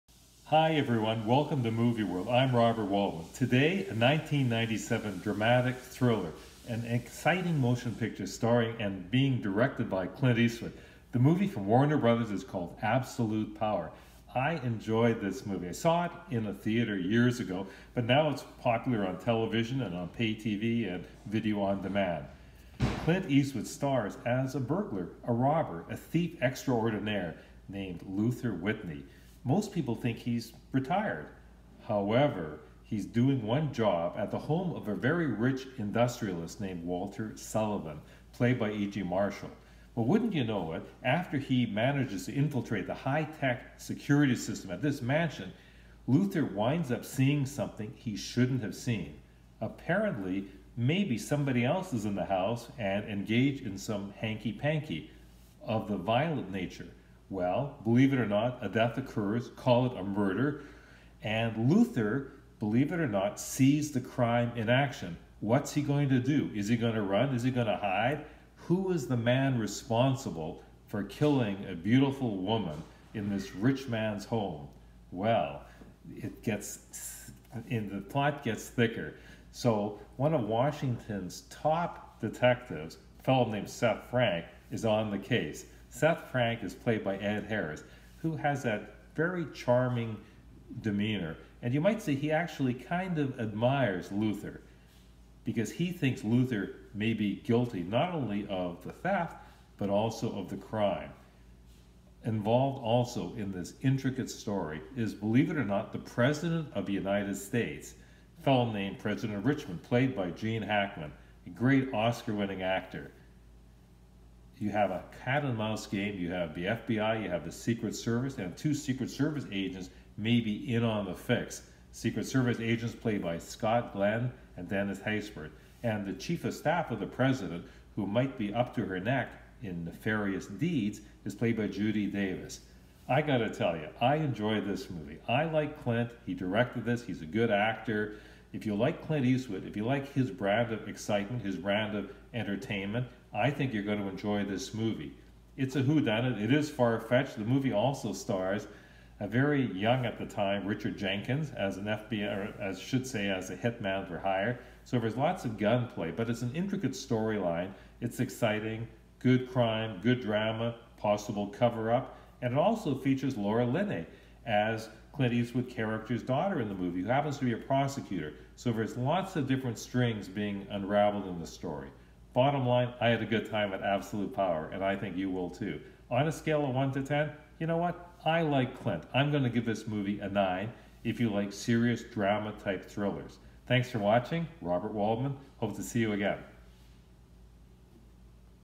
Absolute Power movie review by Robert Waldman The Movie World
The American presidency comes under Fire big time in Absolute Power. Clint Eastwood  perfectly portrays Luther,  a master of disguise jewel thief who catches the main man in The act during a standard break-in, Clint is at the top of his game with Gene Hackman sleazy as ever as The President with Ed Harris charming as a cop out to solve a crime or three.